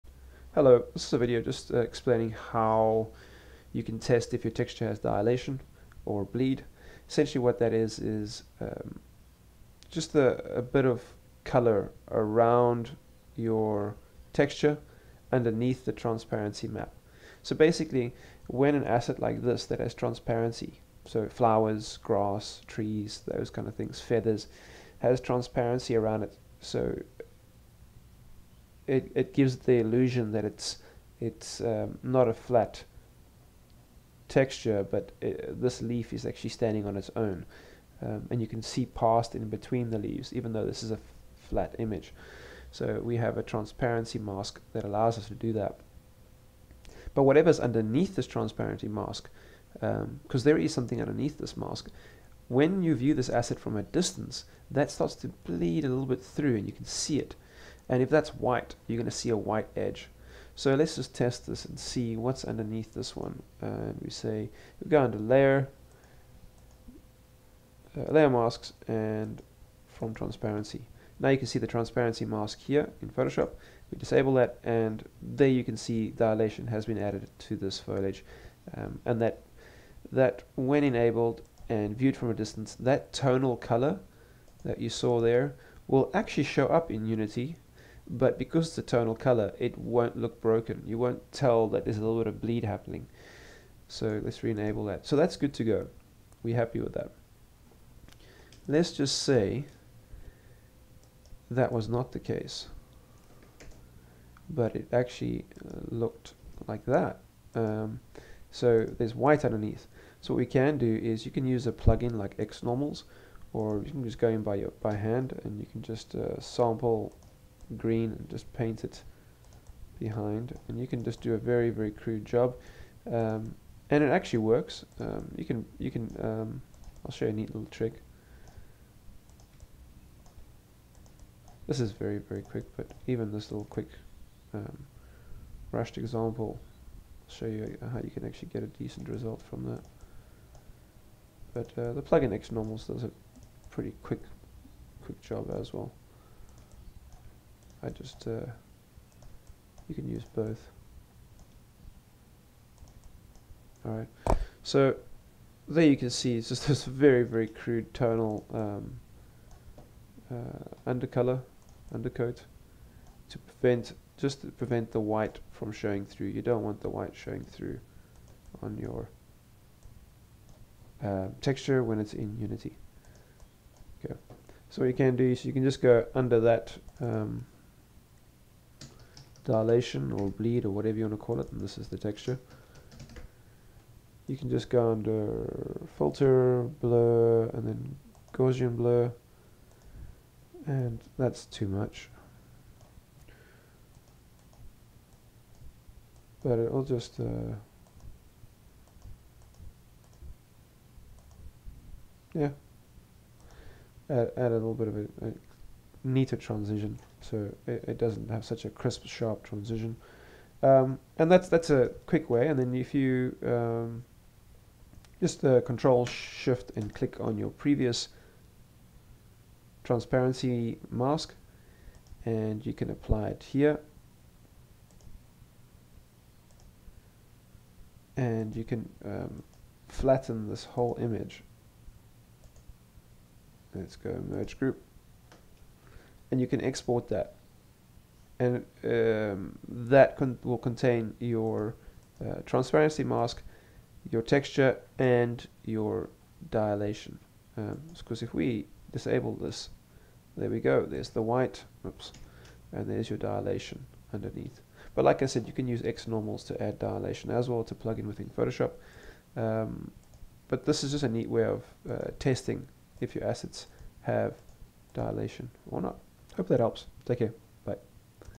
Photoshop - Dilation Bleed
The importance of dilation bleed, how to test your assets to ensure they contain dilation and how to add dilation if they don't.